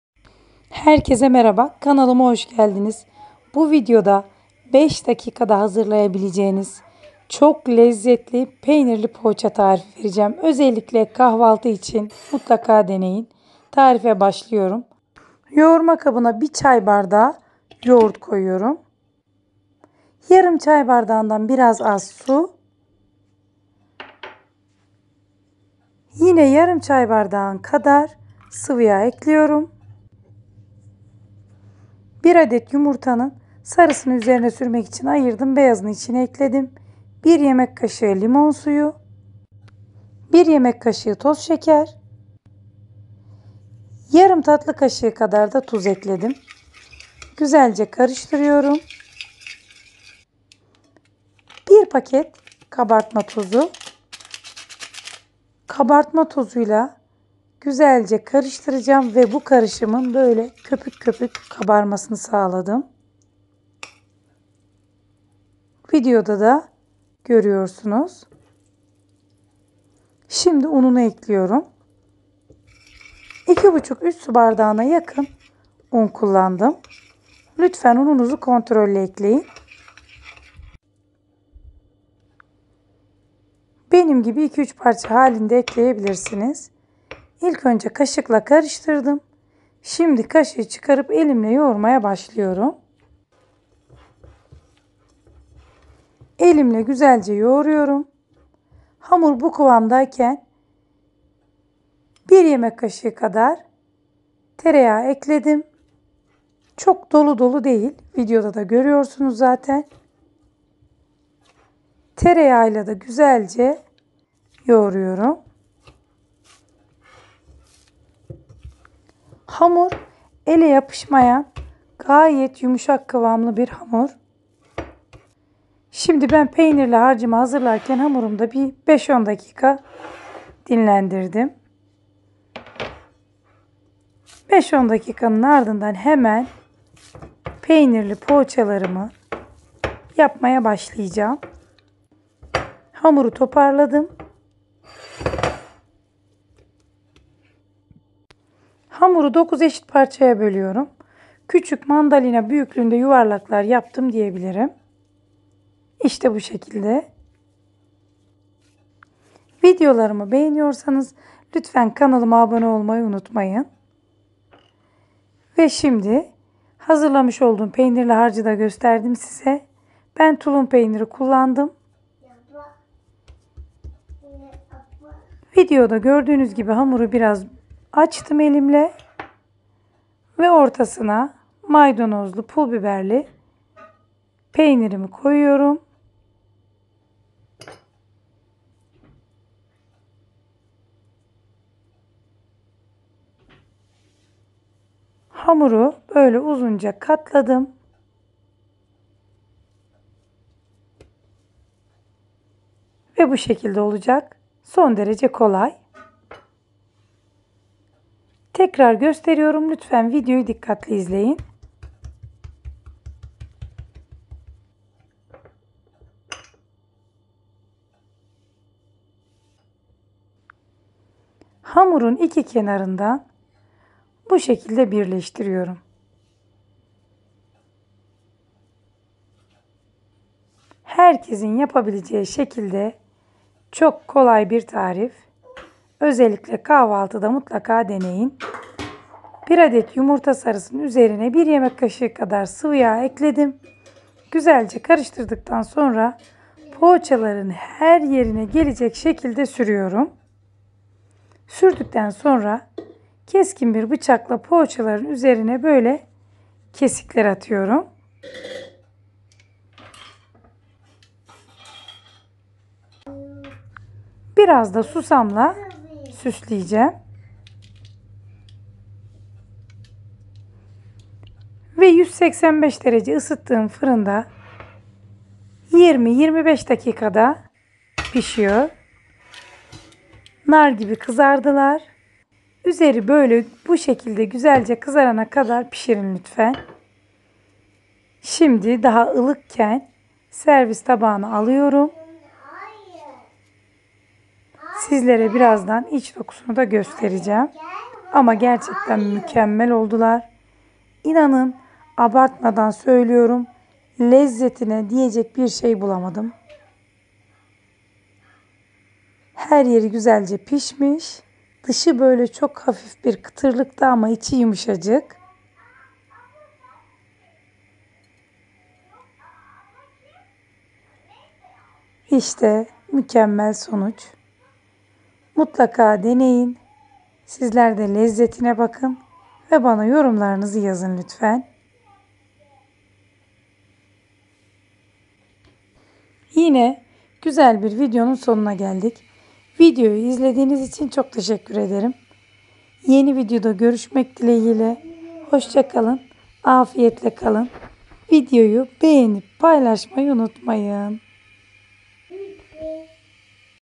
Sadece 10 DAKİKALIK çalışma❗ MÜKEMMEL sonuç🔝MAYASIZ KAHVALTILIK Peynirli Poğaça Tarifi #370
Sadece 10 DAKİKALIK çalışma❗ MÜKEMMEL sonuç🔝MAYASIZ KAHVALTILIK Peynirli Poğaça Tarifi, Mükemmel bir kahvaltı için kabartma tozu ile hızlıca hazır olan lezzetlipeynirli ekmekler👍 Bu kolay poğaça tarifi KAÇMAZ! Kahvaltılık lezzetli poğaça, kahvaltı, yemek tarifleri, enfes lezzet, leziz tarif.

Hızlı Ekmek ( poğaça) Tarifi İçin Malzemeler

120g yoğurt
60ml su
50ml sıvıyağ
1 yumurta beyazı
1 yemek kaşığı limon suyu
1 yemek kaşığı toz şeker
Yarım tatlı kaşığı tuz
10g kabartma tozu
340g un
30g tereyağı

İç Dolgu: Biraz tulum peyniri, maydanoz ve pul biber

Üzerine sürmek için; 1 yumurta sarısı + 1 yemek kaşığı sıvıyağ

Youtube/ Nefis Sanal Lezzetler

Umarım video işinize yarar. Sizin için düzenli olarak yeni tarifler bu kanalda olacak.